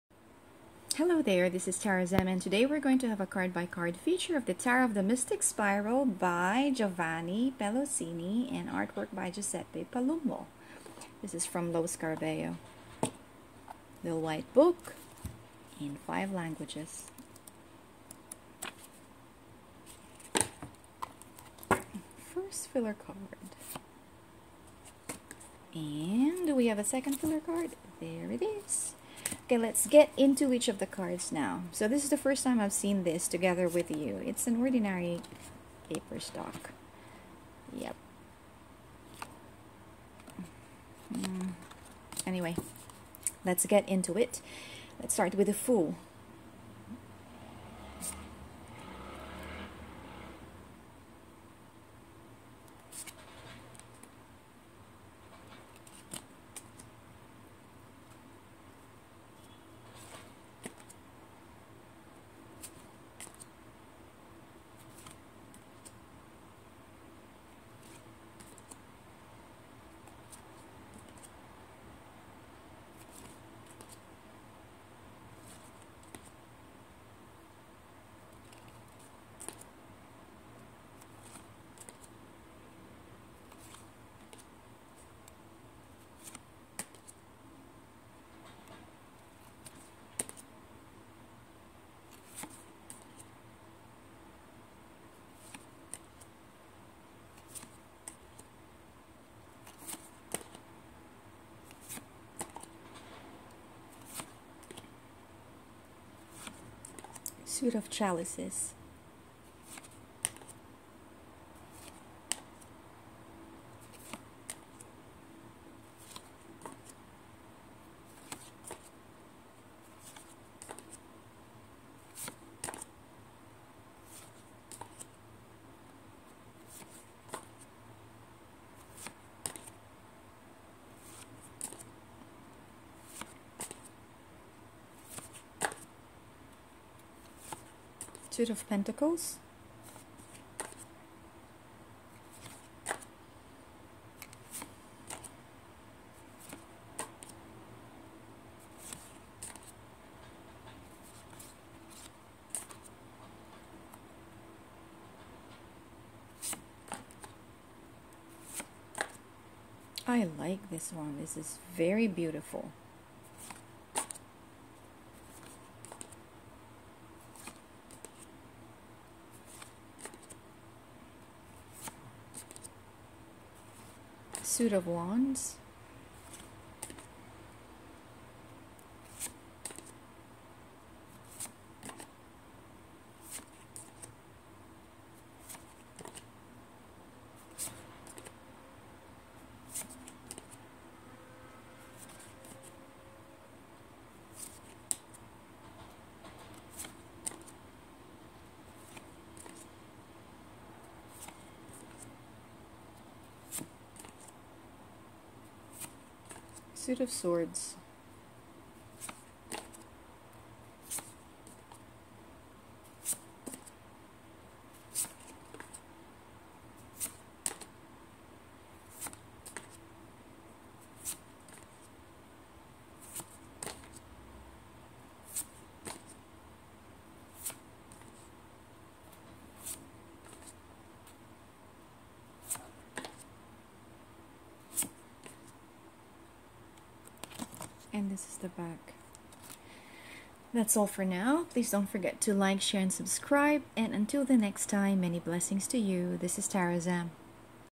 Tarot of the Mystic Spiral
Tarot of the Mystic Spiral by (author)  Giovanni Pelosini, Illustrated by  Giuseppe Palumbo - A card-by-card feature by Tarot Zamm.

The everlasting search for knowledge and wisdom is represented by the spiral. It can be found in the galaxies and in DNA, the sacred geometry used in the construction of the pyramids and gothic cathedrals, mathematical constants, alchemist's formulas, and art dating back to the Stone Age. This mysterious, cosmic symbol is also indicative of a journey, an evolution. Use the Tarot of the Mystic Spiral to help you on your own quest--whether you're seeking personal growth, spiritual enlightenment, or a glimpse into the ultimate meaning of life. Boxed deck (23/4 x 43/4) includes 78 full-color cards and instruction booklet The companion booklets for most Lo Scarabeo decks are in five languages: English, Spanish, French, Italian, and German.

Format Cards
Dimensions 68.58 x 124.46 x 30.48mm | 226.8g
Publication date 08 Jan 2012
Publisher Llewellyn Publications
Publication City/Country United States
Language English
Edition Bilingual edition
Edition statement Cards W/ Instruction Booklet ed.
Illustrations note Illustrations, unspecified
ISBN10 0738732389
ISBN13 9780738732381
Source: Bookdepository.com

Amazon Customer reviews 3.8 out of 5 stars
Goodreads  5.0  ·  Rating details ·  1 Rating